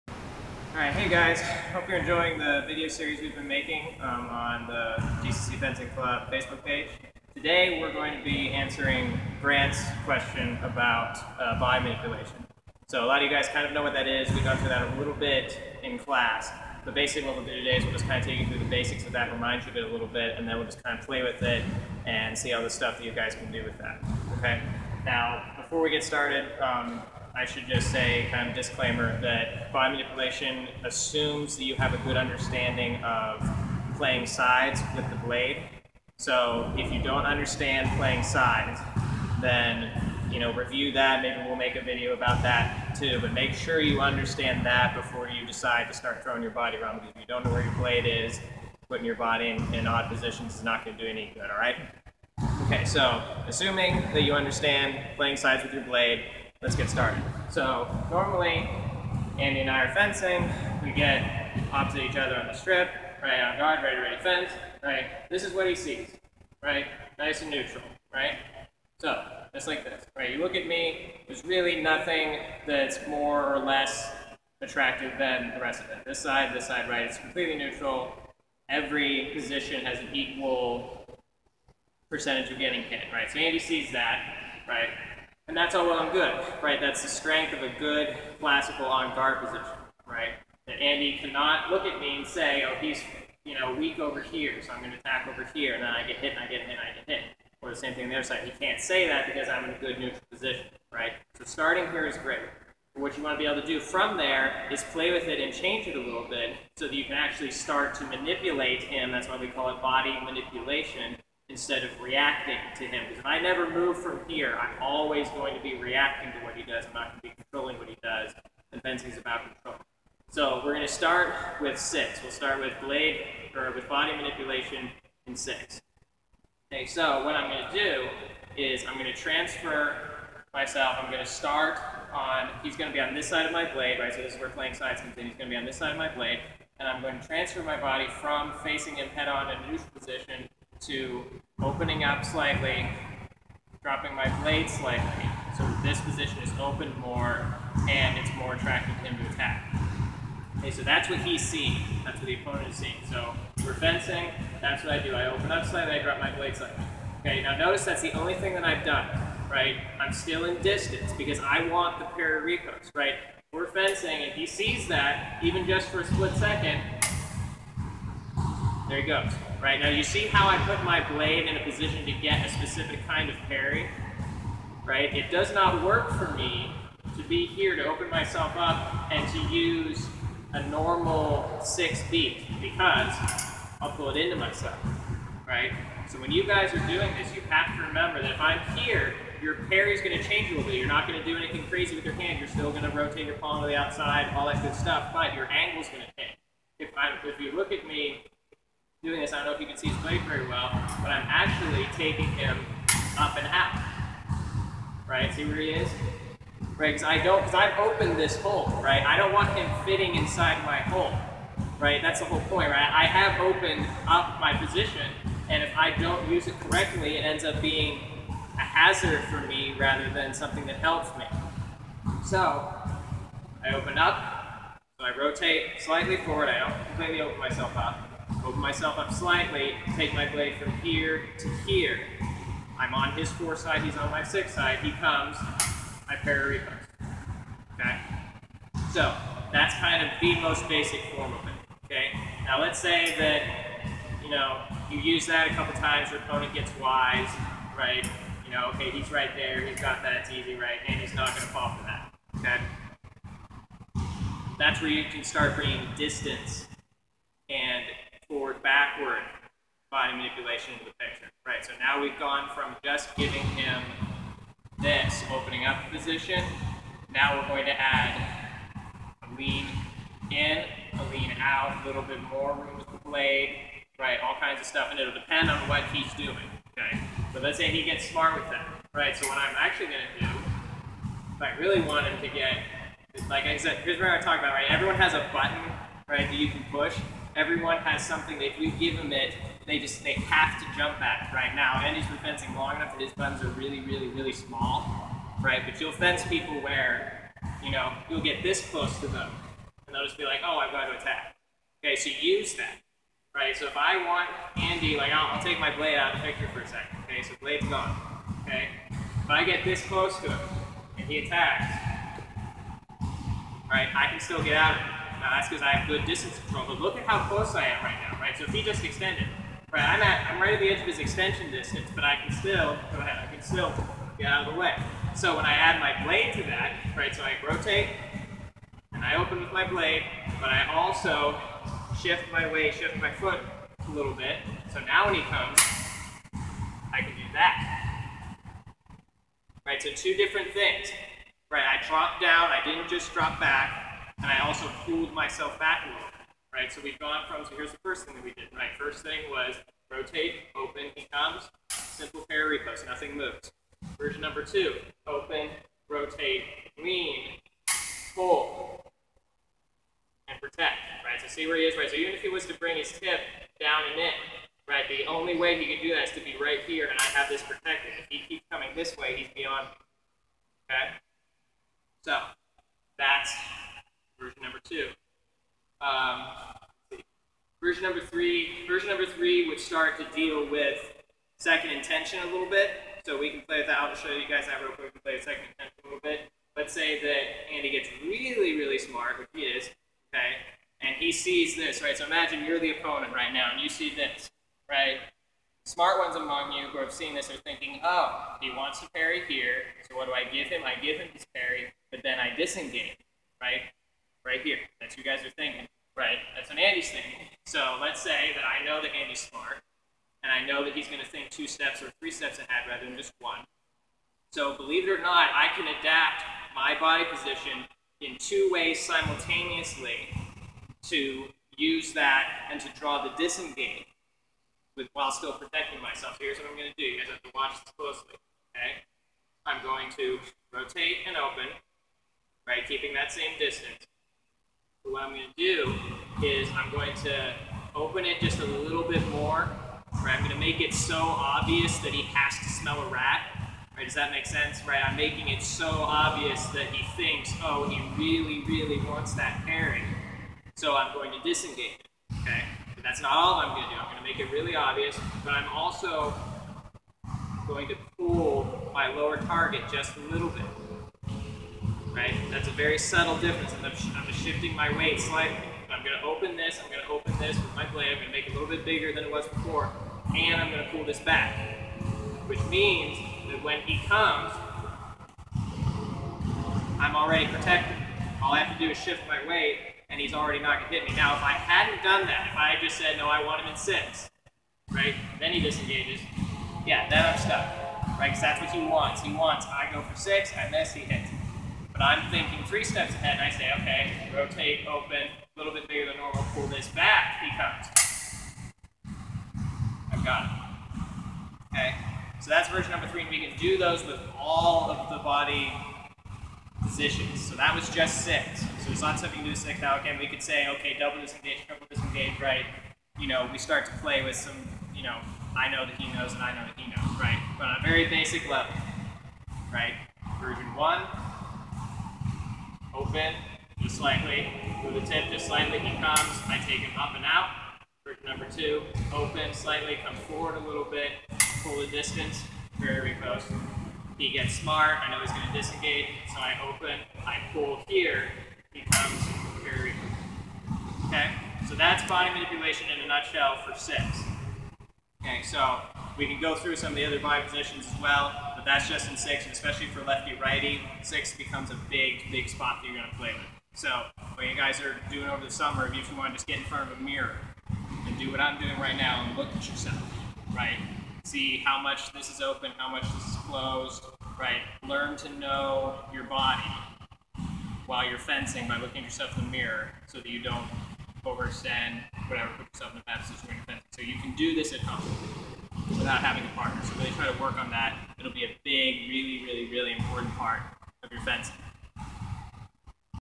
Tactical Workshop Body Manipulation
Instructors Andy Herbener and Caleb Thrasher create a workshop video in Body Manipulation as they delve into the inner aspects of fencing. This is a small introduction into the "physical chess" of the Long Method of fencing.